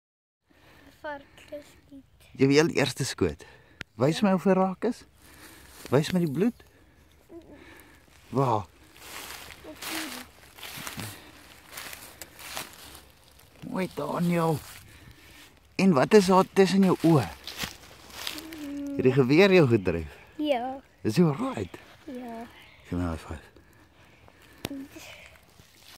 Daniel van Niekerk age 6 shot his first Warthog.. Video 1 of 3
Musgrave 7x57 .. age 6 .. Daniel van Niekerk.. plaas Koedoesberg Pongola